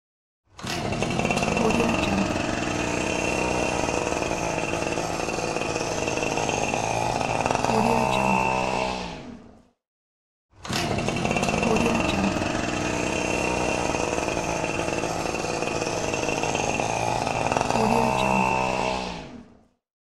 Sound - Mowing the Lawn | AudioJungle Download
Download Sound - Mowing the Lawn | AudioJungle

Category : Domestic Sounds. Sound

Sound of mowing the lawn.